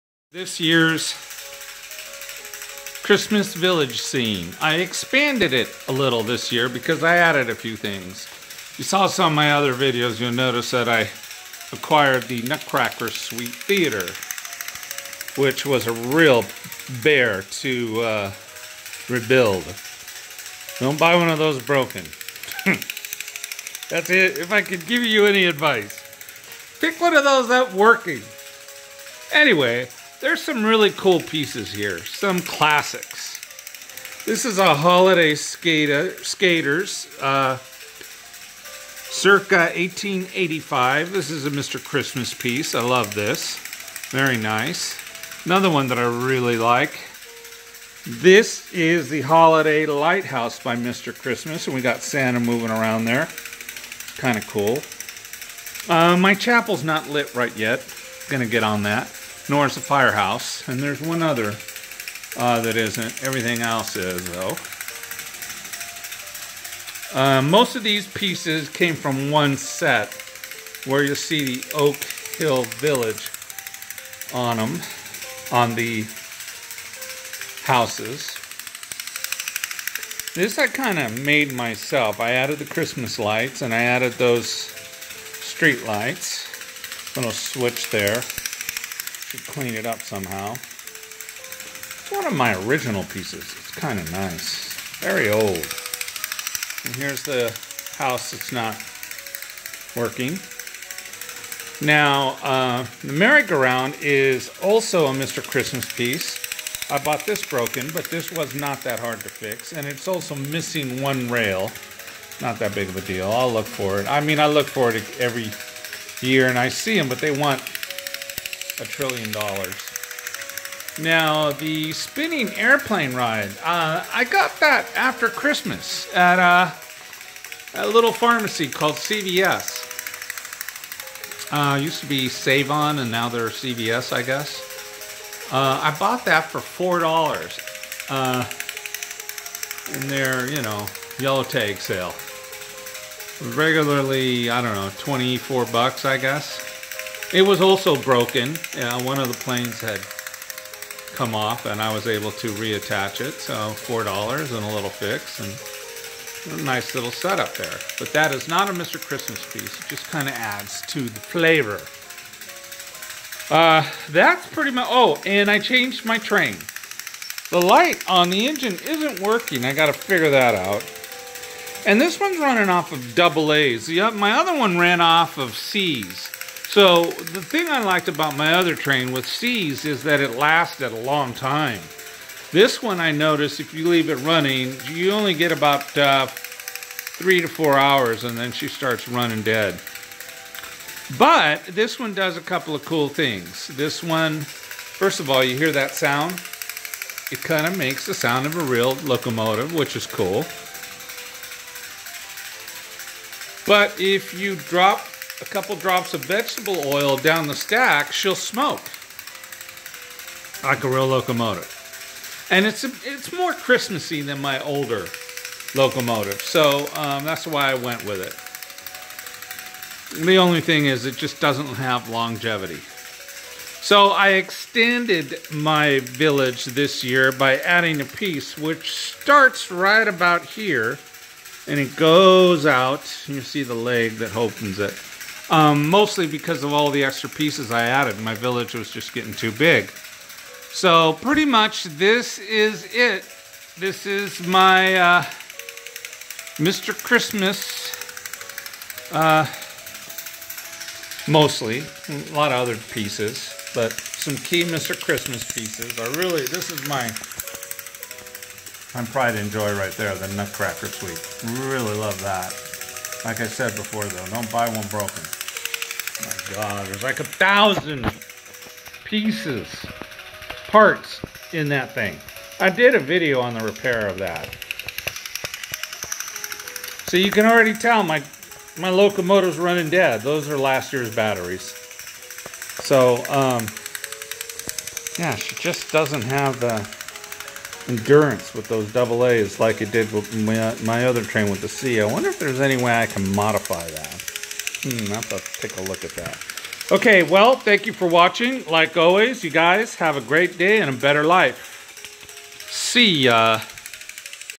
Expanded Oak Hill Christmas Village
Adding a few more pieces & upgrading some others.